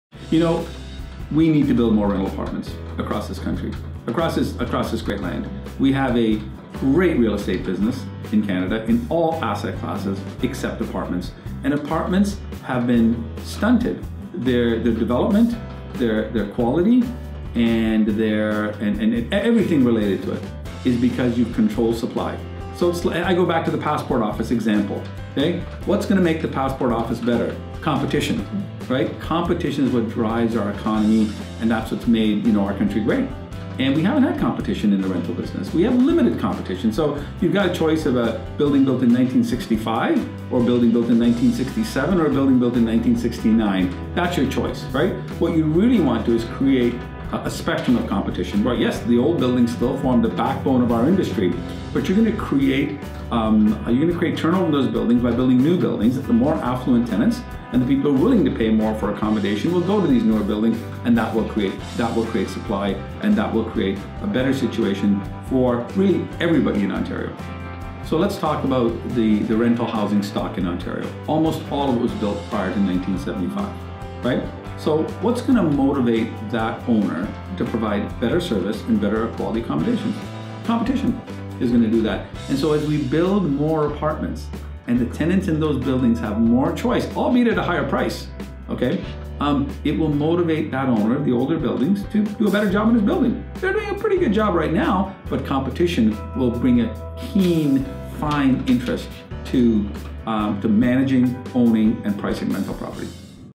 Developers: Keep building rentals!
The new Ontario Fair Housing Plan was brought out so quickly, it's hard to know what it really means for the people of Ontario. Derek Lobo, "The Apartment Expert", unpacks the Fair Housing Plan and what Rent Control will really do.